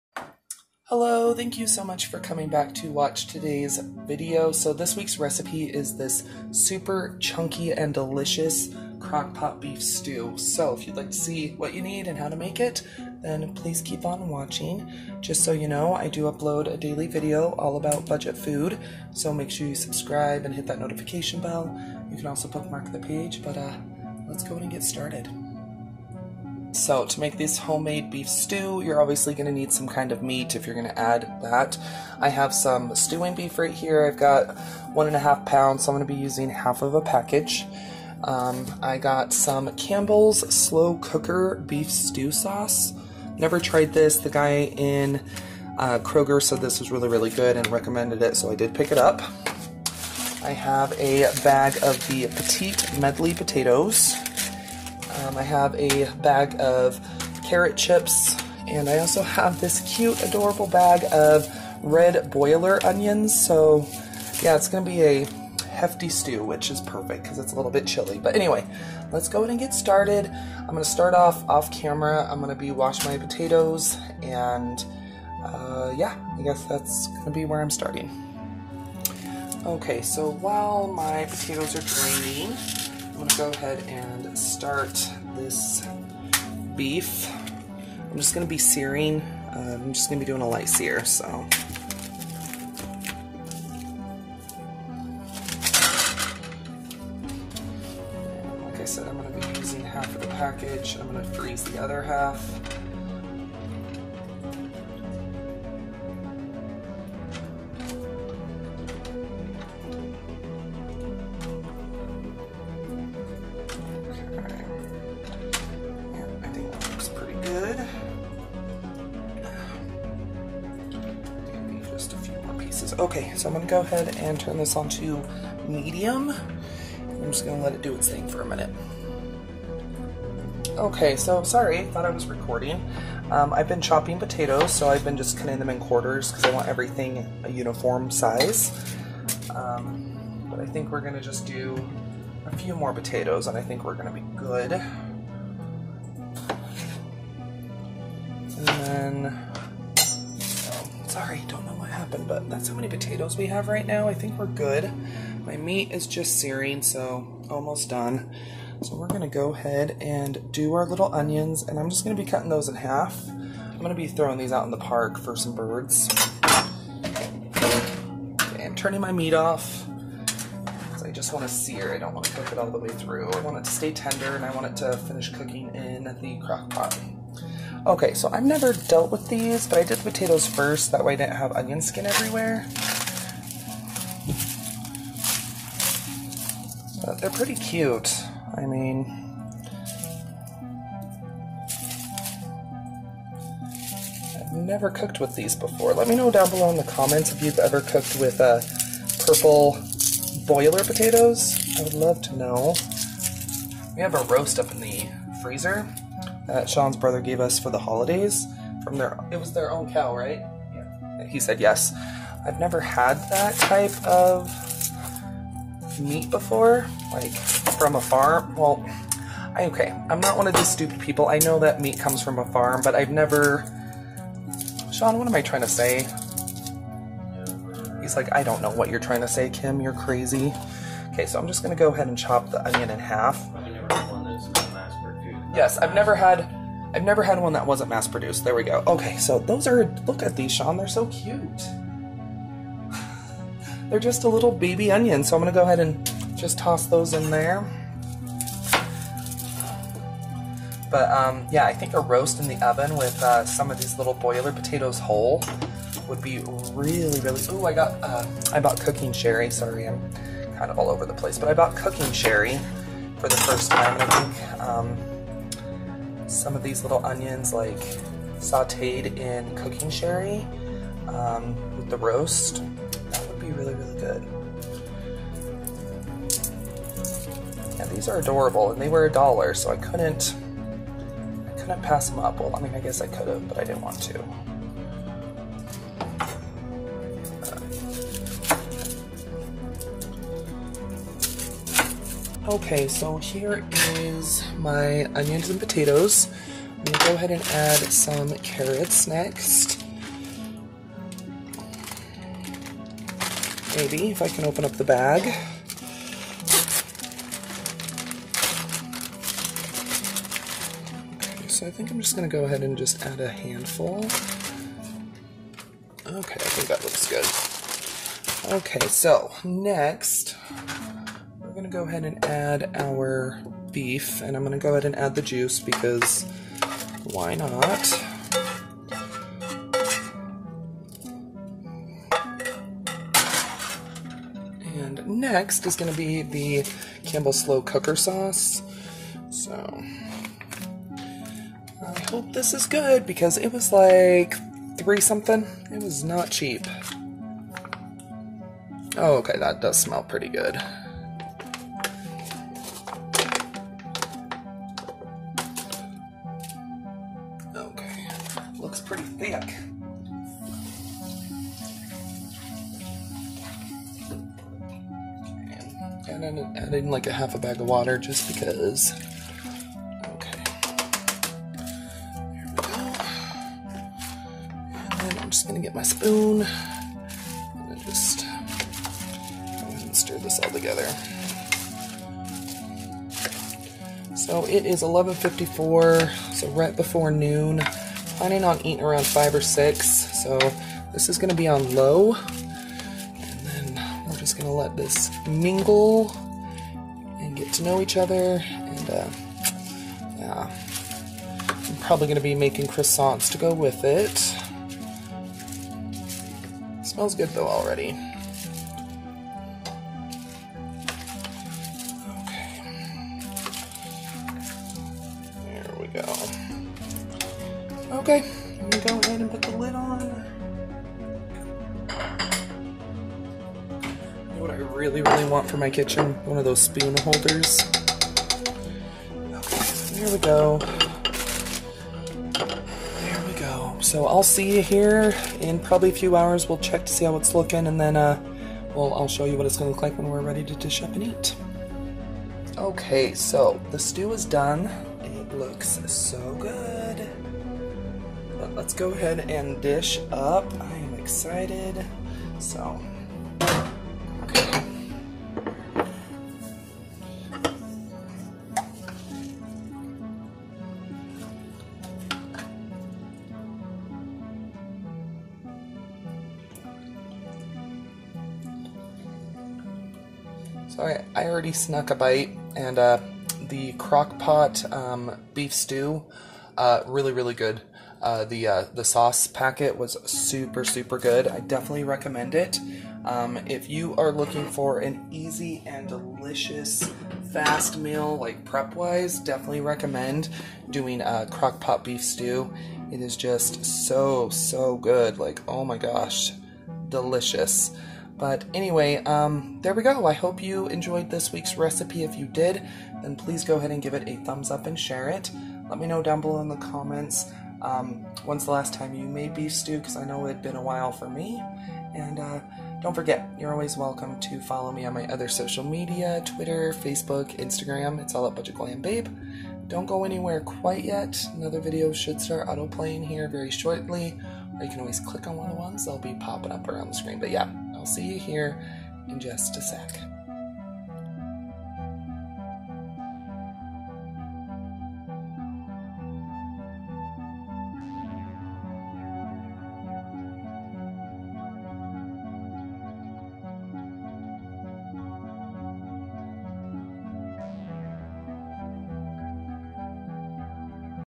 Crock-Pot Beef Stew
This Crock-Pot Beef Stew is super easy and makes a great dinner with plenty of leftovers.